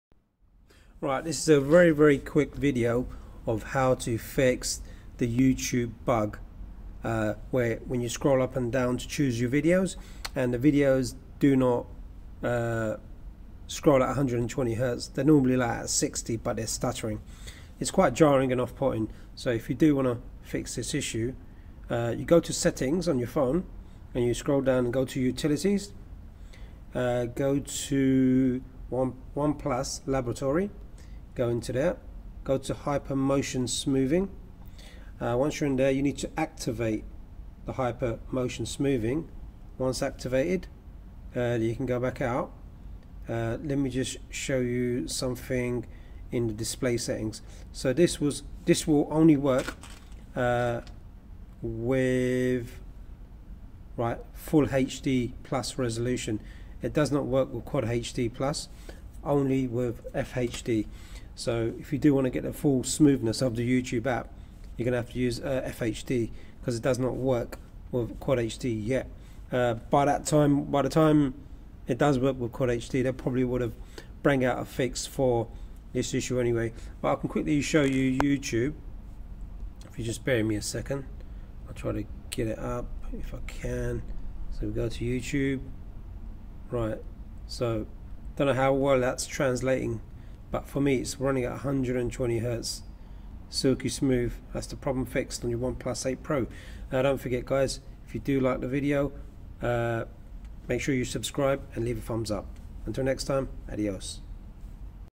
OnePlus 8 Pro Youtube App Stutter/Lag Fix Very Easy
OnePlus 8 Pro YouTube App Stutter/Lag Stuck on 60hz after latest update I found a quick and easy fix which only works in FHD+ and not with QHD, it even works with FHD at 60hz making only the YouTube app run at 120hz strange, but I would just leave 120hz with FHD to make everything smooth hope you enjoy the fix :)